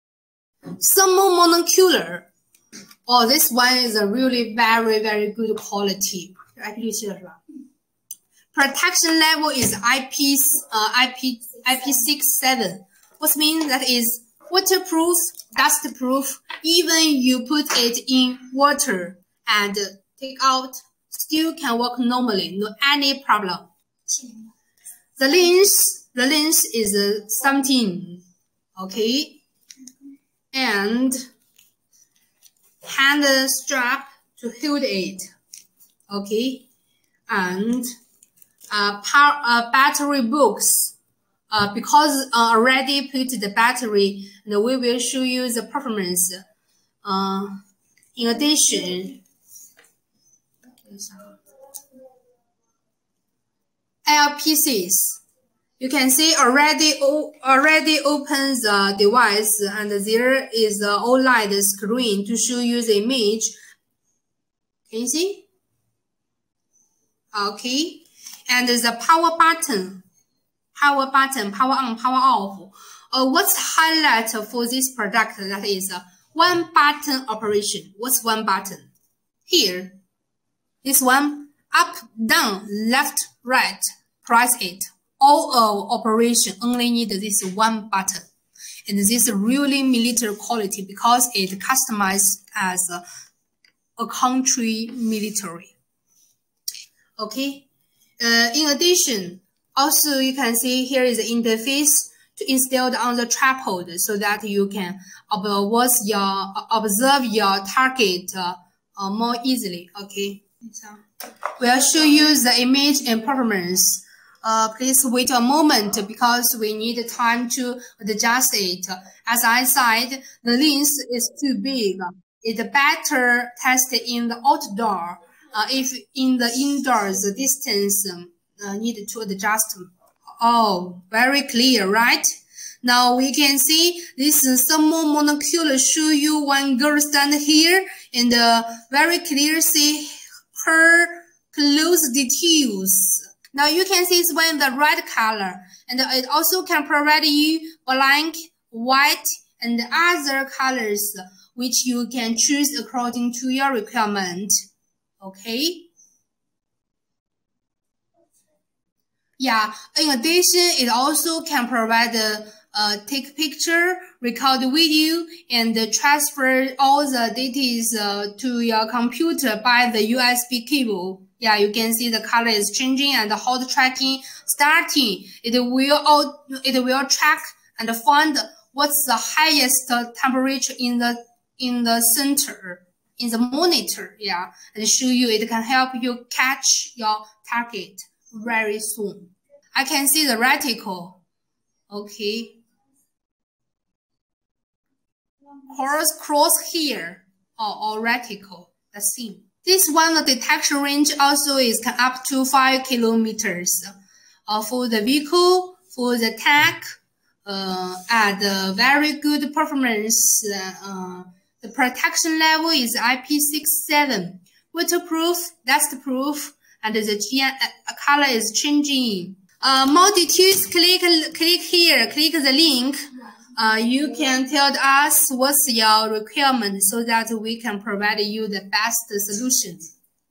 Handheld long range thermal monocular
Military long range thermal monocular

640*512px@12μm thermal detector,70mm lens
Grace